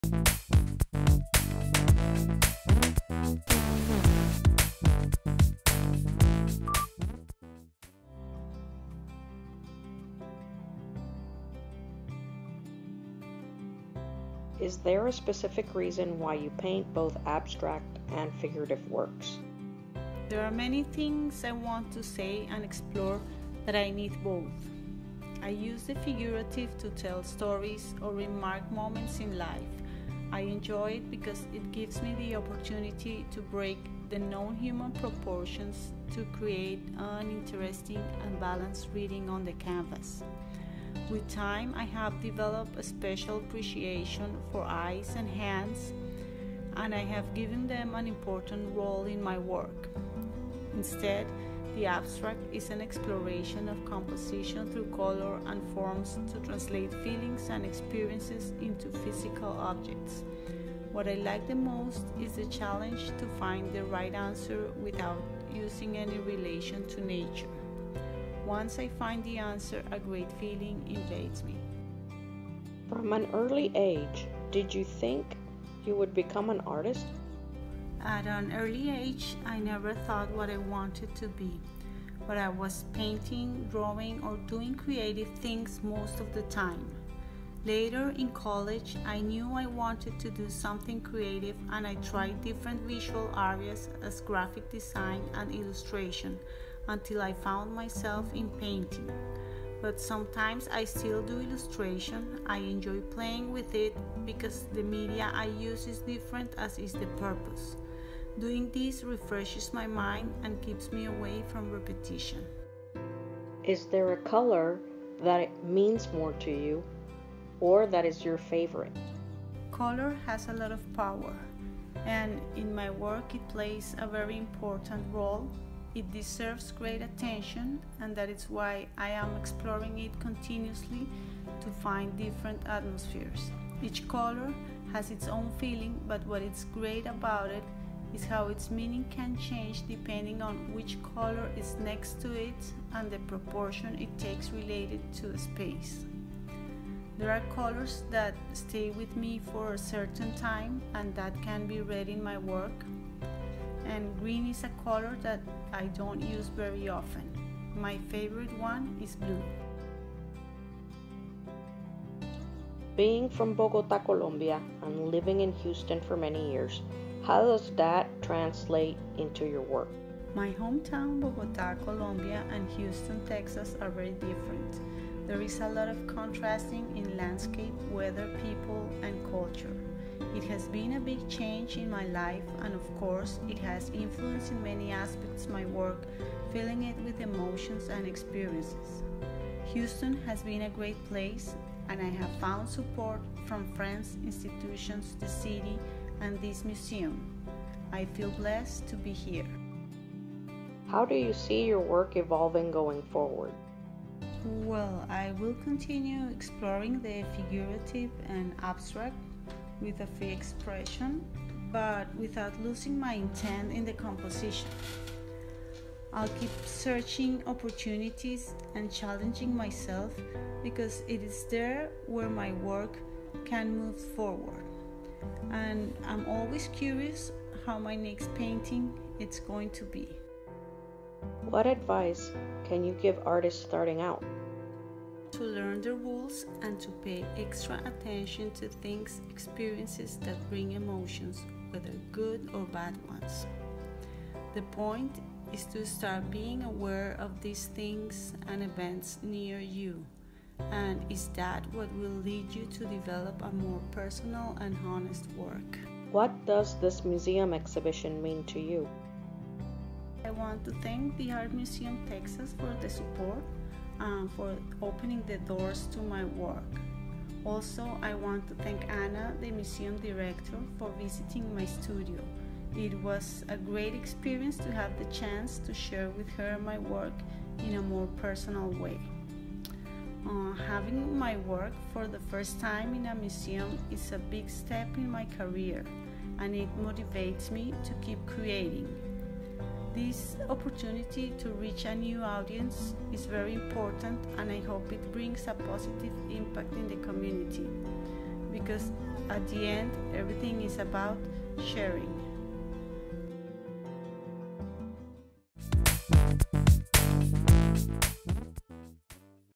Five-Minute Tour: Tatiana Escallon at Art Museum TX, Katy
Tatiana Escallon: "New Walls | New Corners" at Art Museum TX, Katy.
Dates: June 3 - September 27, 2020.

Artists also included in the "New Walls | New Corners" exhibition are Susan Budge, Mary Lynch, Lorena Morales, and Kelley Devine.